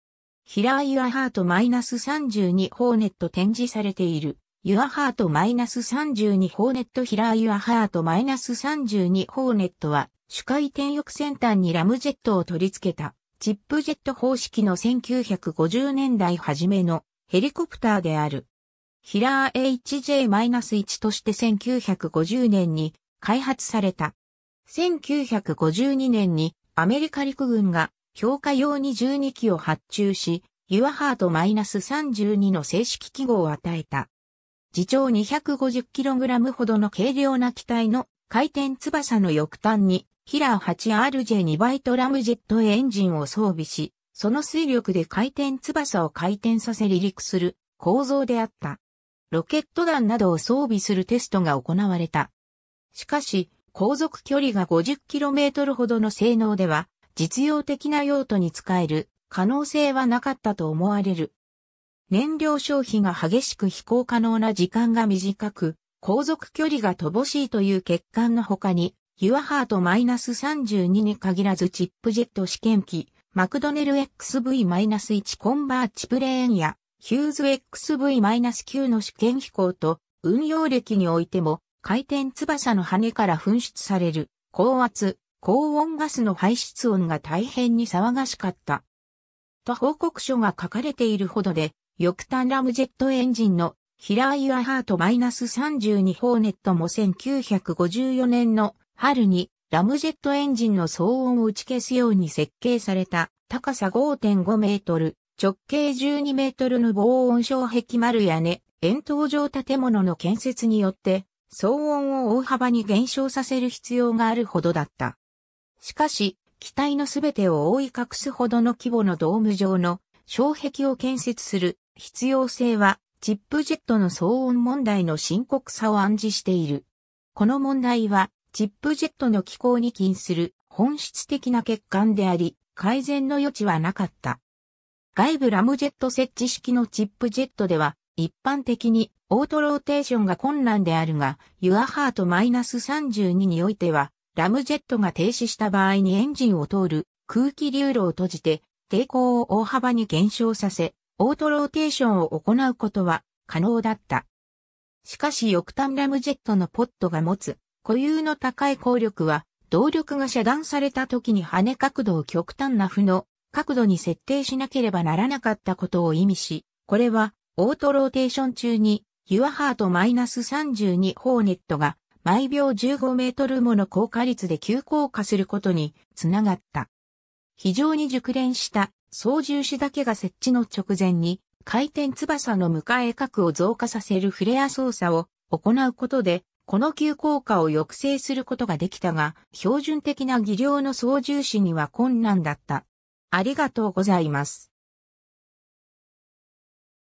YH-32 (航空機)
ヒラー YH-32 ホーネット 展示されているYH-32 ホーネット ヒラーYH-32 ホーネット ( Hiller YH-32 Hornet ) は主回転翼先端にラムジェットを取り付けたチップジェット方式の1950年代始めのヘリコプターである。
 ヒラー HJ-1として1950年に開発された。
1952年にアメリカ陸軍が評価用に12機を発注し、YH-32の制式記号を与えた。
 自重250kg程の軽量な機体の回転翼の翼端にヒラー8RJ2Bラムジェットエンジンを装備し、その推力で回転翼を回転させ離陸する構造であった。
ロケット弾などを装備するテストが行われた。
 しかし、航続距離が50kmほどの性能では実用的な用途に使える可能性はなかったと思われる。
 燃料消費が激しく (燃費が悪い) 飛行可能な時間が短く、航続距離が乏しいという欠陥のほかに、YH-32 に限らずチップジェット試験機 マクドネル XV-1・コンバーチプレーン や、ヒューズ XV-9（ モデル385 ）の試験飛行と運用歴においても、 " 回転翼の羽根から噴出される、高圧・高温ガスの排出音が大変に騒がしかった。
" と報告書が書かれている ほどで、翼端ラムジェットエンジンの「ヒラーYH-32 ホーネット」も 1954年の春に、ラムジェットエンジンの騒音を打ち消すように設計された高さ 5.5 m 、直径 12 m の防音障壁丸屋根・円筒状建物の建設によって騒音を大幅に減少させる必要があるほどだった。
 しかし機体の全てを覆い隠すほどの規模のドーム状の障壁を建設する必要性は、チップジェットの騒音問題の深刻さを暗示している。
 この問題はチップジェットの機構に起因する本質的な欠陥であり、改善の余地は無かった。
 外部ラムジェット設置式のチップジェットでは、一般的にオートローテーションが困難であるが、YH-32 においては、ラムジェットが停止した場合にエンジンを通る空気流路を閉じて抵抗を大幅に減少させ、オートローテーションを行うことは可能だった。
 しかし翼端ラムジェットのポッドが持つ固有の高い抗力は、動力が遮断されたときに羽根角度を（従来型のヘリコプターと比較して）極端な負の角度に設定しなければならなかったことを意味し、これはオートローテーション中に YH-32 ホーネットが 毎秒15 m もの降下率で急降下することにつながった。
 非常に熟練した操縦士だけが接地の直前に回転翼の迎角を増加させるフレア操作を行うことで、この急降下を抑制することが出来たが、標準的な技量の操縦士には困難だった。